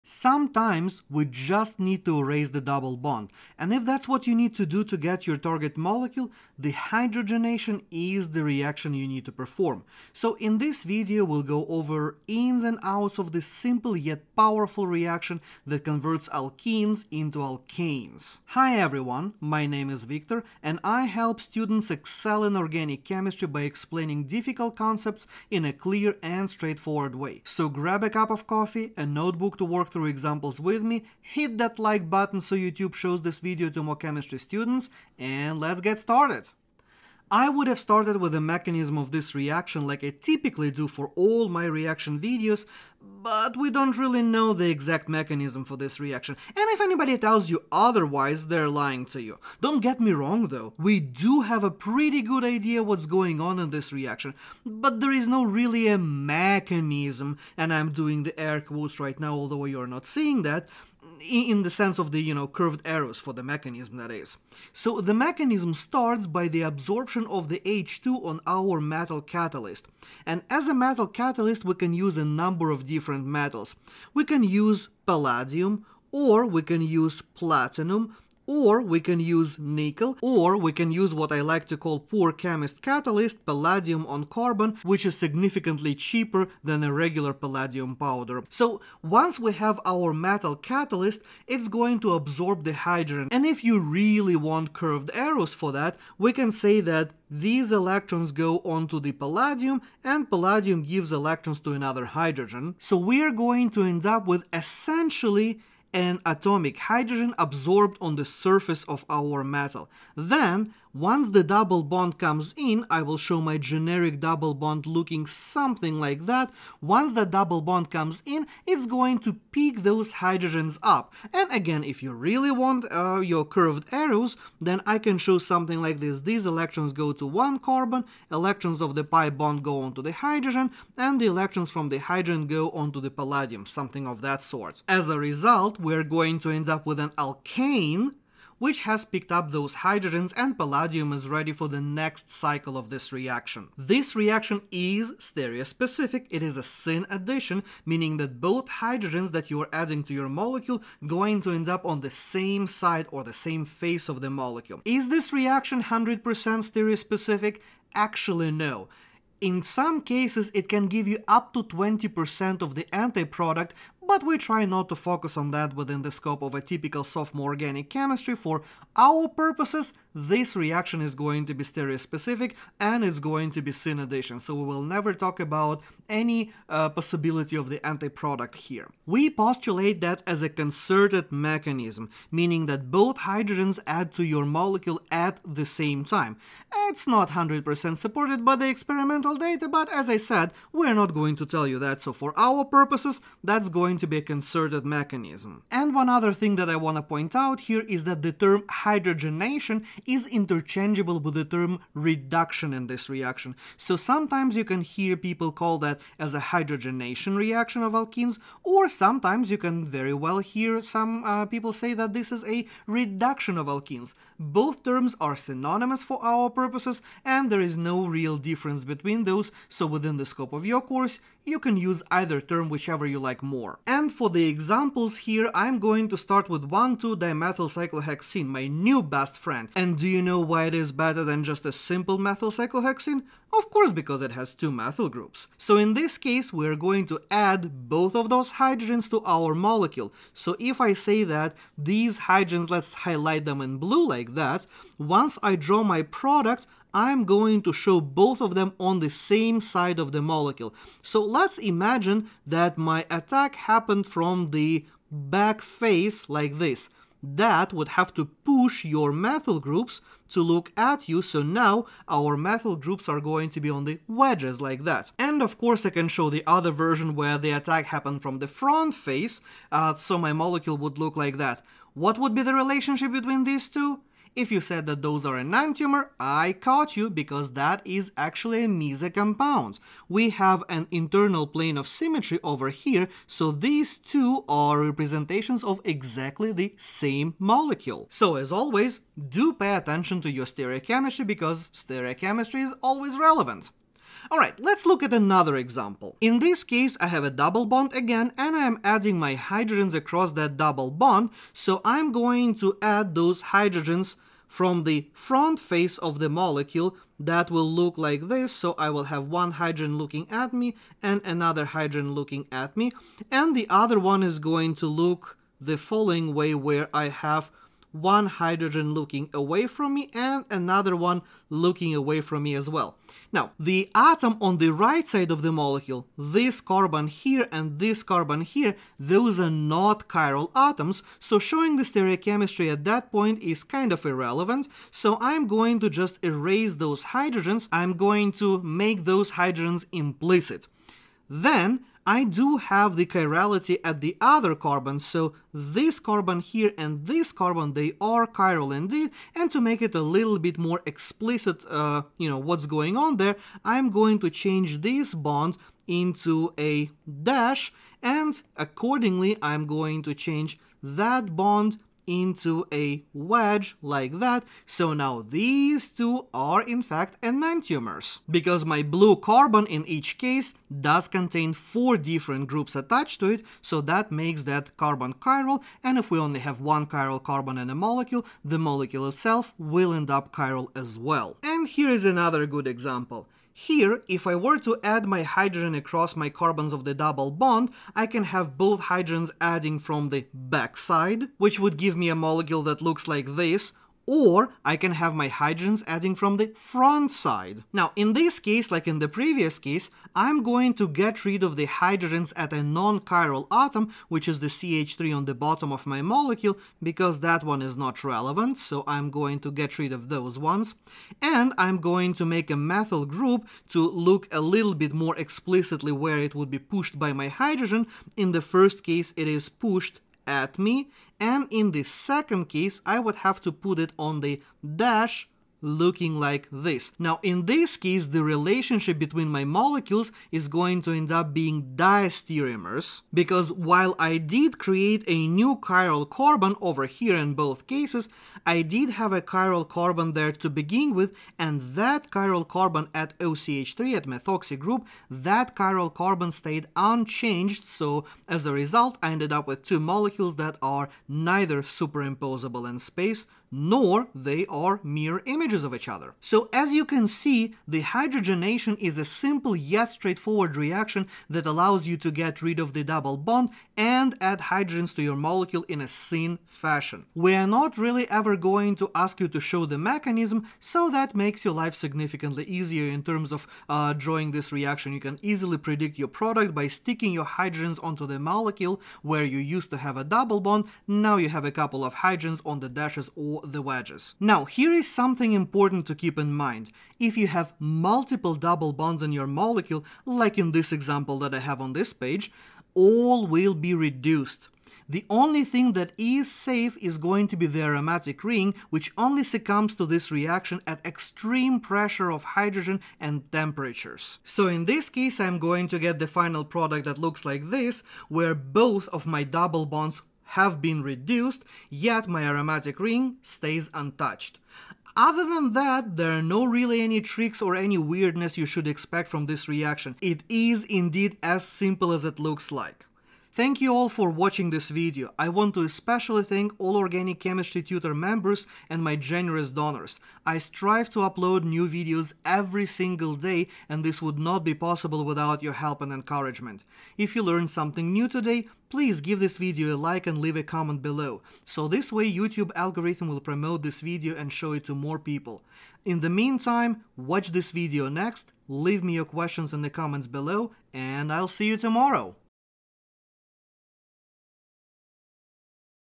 Hydrogenation (Reduction) of Alkenes = Double Bond Eraser
Hydrogenation of alkenes is you premiere way of getting rid of the double bonds. In this video, we'll talk about how this reaction works and what tricks to expect on the test.

00:00 Intro and "Mechanism" of the Hydrogenation
03:43 Example 1
04:56 Example 2
06:21 Example 3
08:04 How to Deal with Multiple Double Bonds